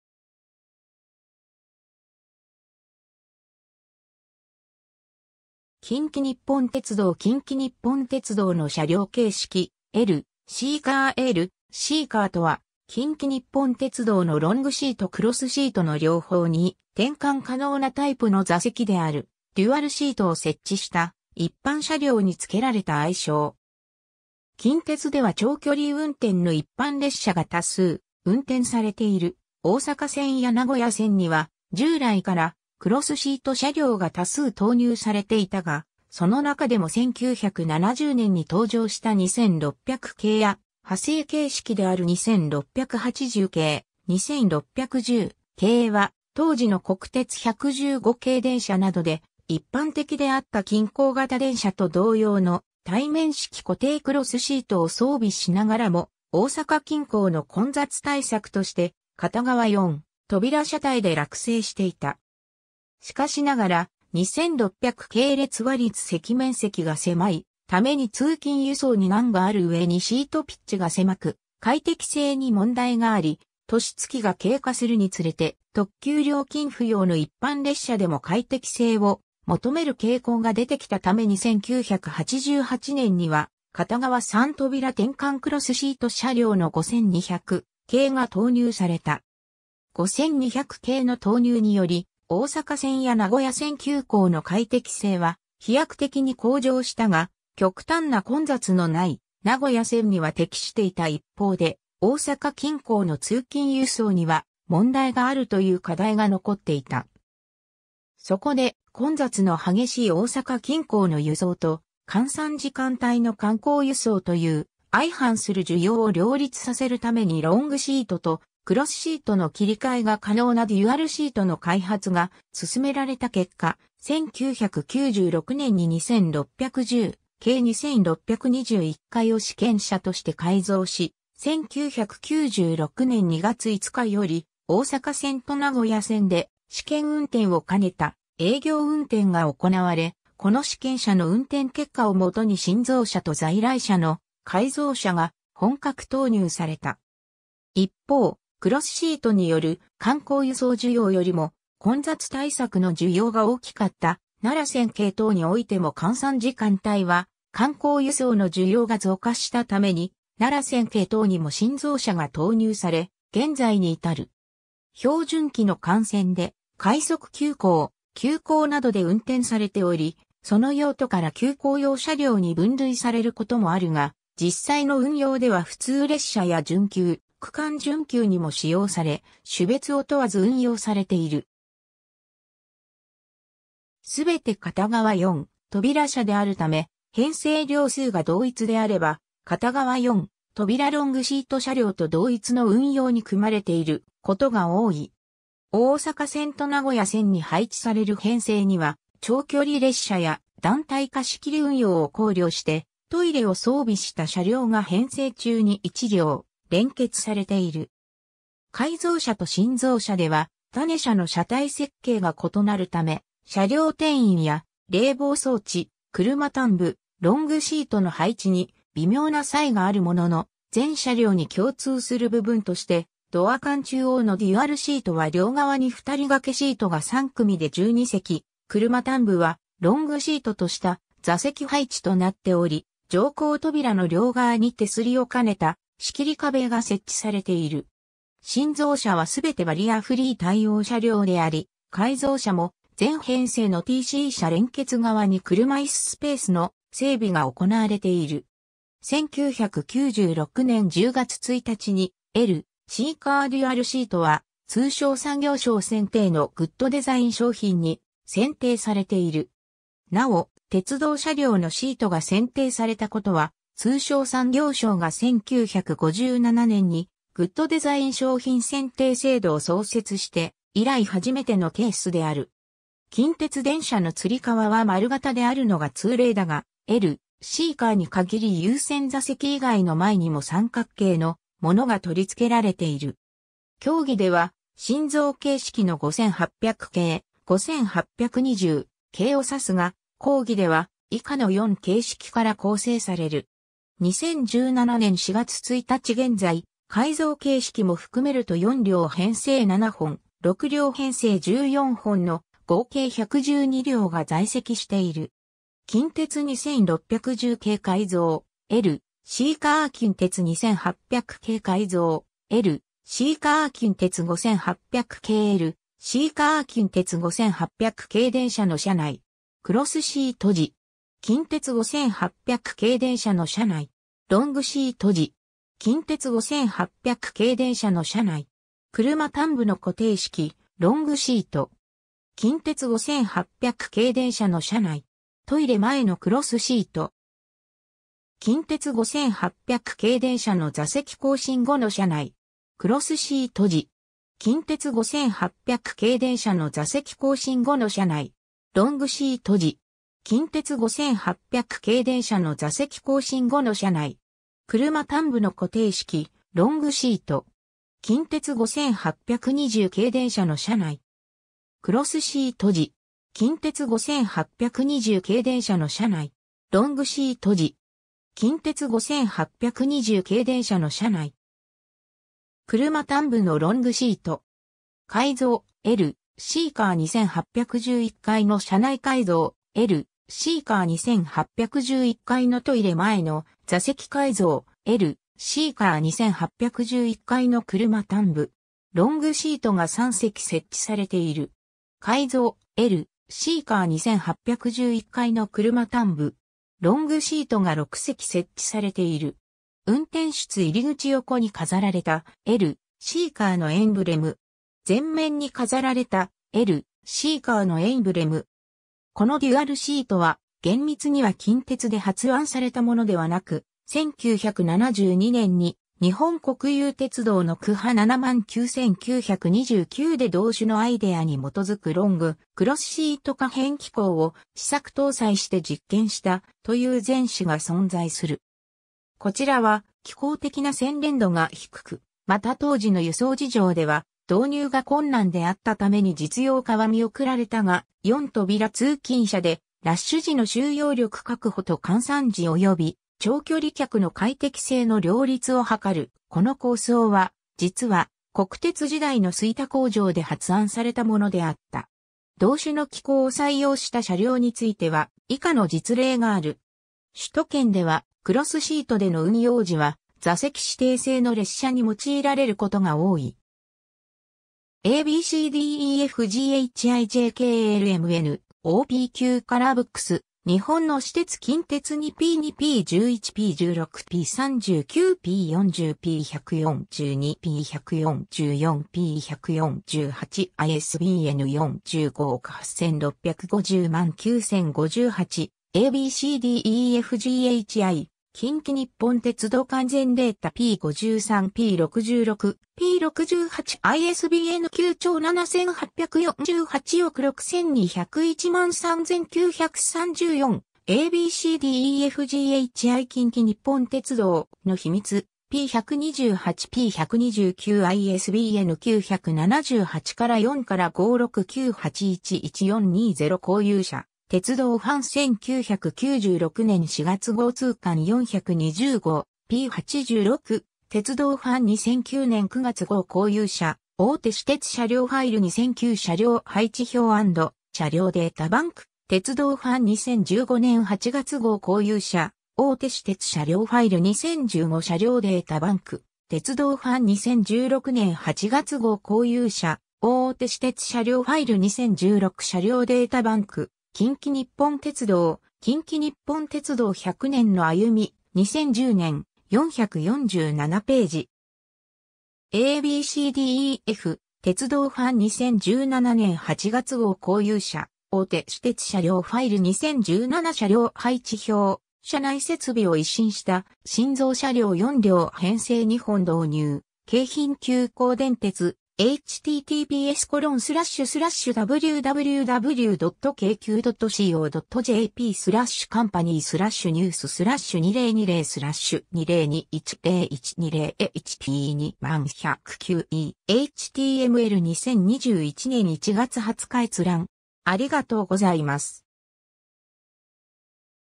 L/Cカー
近畿日本鉄道 」 近畿日本鉄道の車両形式 」 L/Cカー L/Cカー（LC Car）とは、近畿日本鉄道（近鉄）のロングシート・クロスシートの両方に転換可能なタイプの座席であるデュアルシートを設置した一般車両につけられた愛称。
 近鉄では長距離運転の一般列車が多数運転されている大阪線や名古屋線には従来からクロスシート車両が多数投入されていたが、その中でも1970年に登場した2600系や派生形式である2680系・2610系は当時の国鉄115系電車などで一般的であった近郊型電車と同様の対面式固定クロスシートを装備しながらも、大阪近郊の混雑対策として片側4扉車体で落成していた。
しかしながら、2600系列は立席面積が狭いために通勤輸送に難がある上にシートピッチが狭く快適性に問題があり、年月が経過するにつれて特急料金不要の一般列車でも快適性を求める傾向が出てきたために1988年には片側3扉転換クロスシート車両の5200系が投入された。
 5200系の投入により、大阪線や名古屋線急行の快適性は飛躍的に向上したが、極端な混雑の無い名古屋線には適していた一方で大阪近郊の通勤輸送には問題があるという課題が残っていた。
そこで、混雑の激しい大阪近郊の輸送と閑散時間帯の観光輸送という相反する需要を両立させるためにロングシートとクロスシートの切替が可能なデュアルシートの開発が進められた結果、1996年に2610系2621Fを試験車として改造し、1996年2月5日より大阪線と名古屋線で試験運転を兼ねた営業運転が行われ、この試験車の運転結果を元に新造車と在来車の改造車が本格投入された。
一方、クロスシートによる観光輸送需要よりも混雑対策の需要が大きかった奈良線系統においても閑散時間帯は観光輸送の需要が増加したために奈良線系統にも新造車が投入され、現在に至る。
 標準軌の幹線で快速急行・急行などで運転されており、その用途から急行用車両に分類されることもあるが、実際の運用では普通列車や準急・区間準急にも使用され、種別を問わず運用されている。
全て片側4扉車であるため、編成両数が同一であれば片側4扉ロングシート車両と同一の運用に組まれていることが多い。
大阪線と名古屋線に配置される編成には長距離列車や団体貸切運用を考慮してトイレを装備した車両が編成中に1両（大阪線所属の5820系は2両）連結されている。
 改造車と新造車では種車の車体設計が異なるため、車両定員や冷房装置、車端部ロングシートの配置に微妙な差異があるものの、全車両に共通する部分としてドア間中央のデュアルシートは両側に2人掛けシートが3組で12席、車端部はロングシートとした座席配置となっており、乗降扉の両側に手すりを兼ねた仕切り壁が設置されている。
新造車は全てバリアフリー対応車両（大阪線所属の5820系は車椅子対応トイレも装備）であり、改造車も全編成のTc車連結側に車椅子スペースの整備が行われている。
 1996年10月1日に「L/Cカー・デュアルシート」は通商産業省選定のグッドデザイン商品（輸送機器部門）に選定されている。
なお、鉄道車両のシートが選定されたことは、通商産業省が1957年にグッドデザイン（Gマーク）商品選定制度を創設して以来初めてのケースである。
 近鉄電車のつり革は丸型であるのが通例だが、L/Cカーに限り優先座席以外の前にも三角形のものが取り付けられている。
 狭義では新造形式の5800系・5820系を指すが、広義では以下の4形式から構成される。
2017年4月1日現在、改造形式も含めると4両編成7本（28両）、6両編成14本（84両）の合計112両が在籍している。
 近鉄2610系改造L/Cカー 近鉄2800系改造L/Cカー 近鉄5800系L/Cカー 近鉄5800系電車の車内（登場時）。
クロスシート時。
 近鉄5800系電車の車内（登場時）。
ロングシート時。
車端部の固定式ロングシート。
トイレ前のクロスシート。
 近鉄5800系電車の座席更新後の車内。
クロスシート時。
ロングシート時。
 近鉄5820系電車の車内。
クロスシート時。
ロングシート時。
車端部のロングシート。
 改造L/Cカー2811Fの...